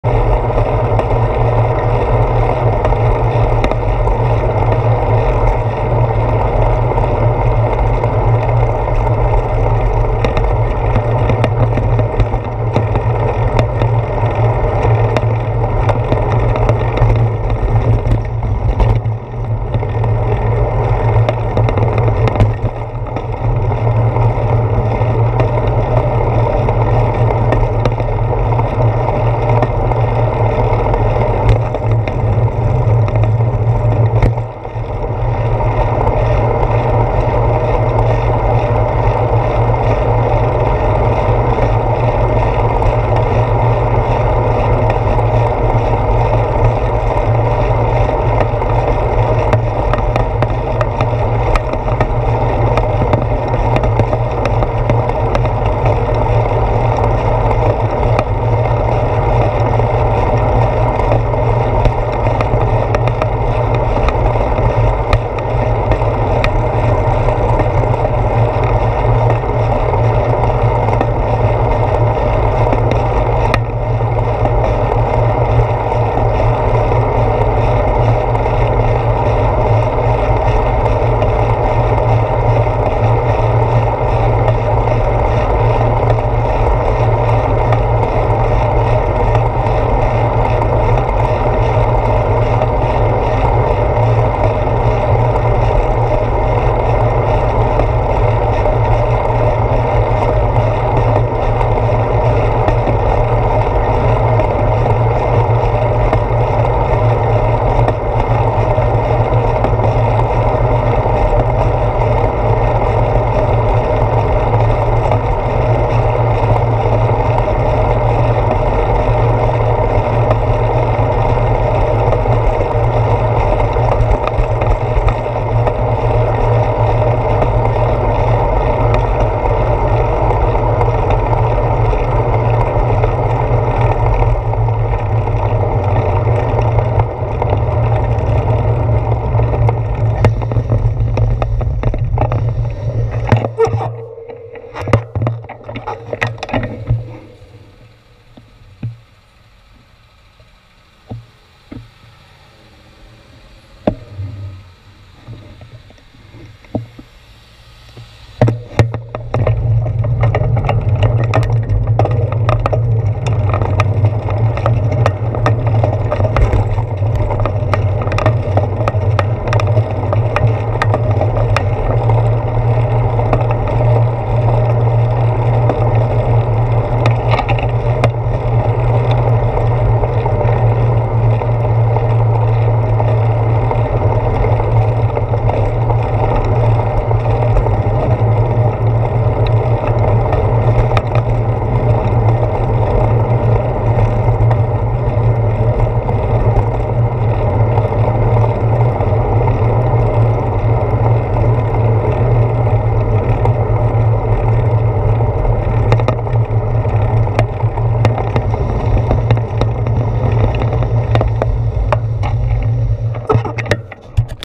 Florida Bike commute Wedneasday morning - Quiet & Humid - front camera, part 1/3
A quiet and humid morning.   There was little to no breeze in this part of Florida, save what I created by biking through the streets of Safety Harbor and Clearwater, Florida on the way to work on the morning of August 26, 2015.